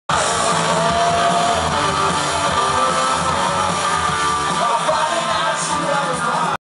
Ronnie Vannucci with Louis XIV at the Cosmo (Segment)
Small segment of RV joining Louis XIV on December 29, 2012 in Las Vegas